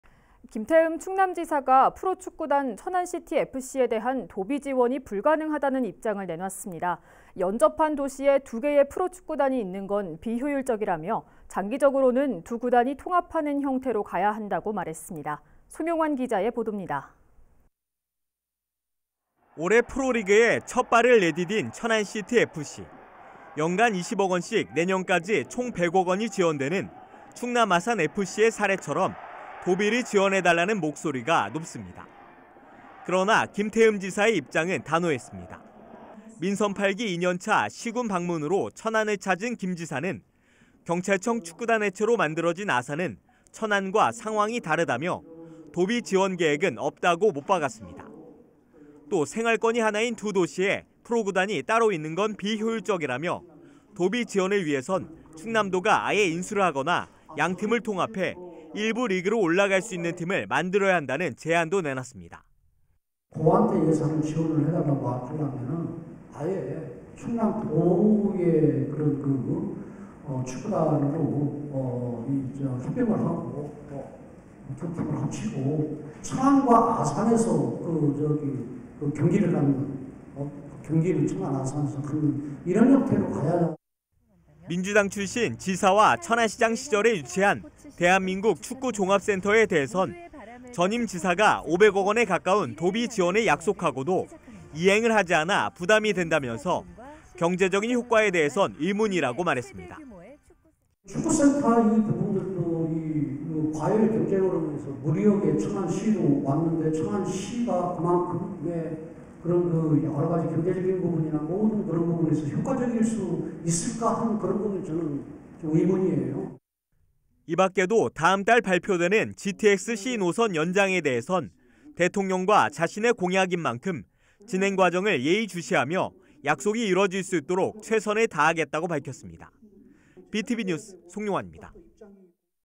김태흠 지사, 천안시티FC 도비 지원 ‘불가’
올해 프로리그에 첫 발을 내디딘 천안시티FC.

연간 20억 원씩 내년까지 총 100억 원이 지원되는
충남아산FC의 사례처럼 도비를 지원해달라는 목소리가 높습니다.

그러나 김태흠 지사의 입장은 단호했습니다.

민선 8기 2년차 시·군 방문으로 천안을 찾은 김 지사는
“경찰청 축구단 해체로 만들어진 아산은 천안과 상황이 다르다”며,
“도비 지원 계획은 없다”고 못 박았습니다.

또 “생활권이 하나인 두 도시에 프로구단이 따로 있는 건 비효율적”이라며
“도비 지원을 위해선 충남도가 아예 인수를 하거나 양 팀을 통합해
1부 리그로 올라갈 수 있는 팀을 만들어야 한다”는 제안도 내놨습니다.

김태흠/충남지사
“충남도에 예산을 지원해달라고 한다면
아예 충남도의 축구단으로 합병을 하고,
두 팀을 합치고 천안과 아산에서 경기를 여는 이런 형태로 가야...”

민주당 출신 지사와 천안시장 시절에 유치한
대한민국 축구종합센터에 대해선
“전임 지사가 500억 원에 가까운 도비 지원을 약속하고도
이행을 하지 않아 부담이 된다”면서
“경제적인 효과에 대해선 의문”이라고 말했습니다.

김태흠/충남지사
“축구센터 이 부분도 과열경쟁으로 인해서 무리하게 천안으로 왔는데,
천안시가 그만큼의 경제적인 부분이나 모든 부분에서
효과적일 수 있을지는 의문이에요.”

이밖에도 다음 달 발표되는 GTX-C 노선 연장에 대해선
대통령과 자신의 공약인 만큼, 진행과정을 예의주시하며
약속이 이뤄질 수 있도록 최선을 다하겠다고 밝혔습니다.

B tv 뉴스 송용완입니다.